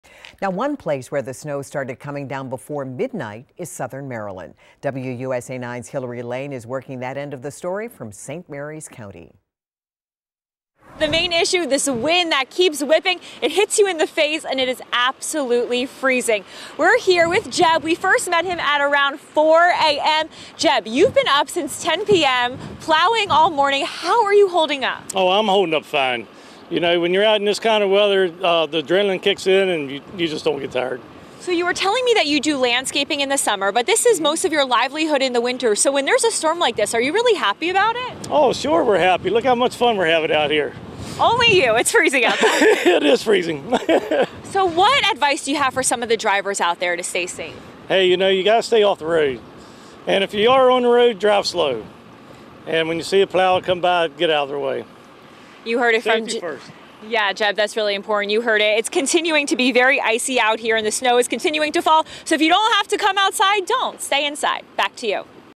Tracking winter conditions in Southern Md.
One place where the snow started coming down before midnight is southern Maryland.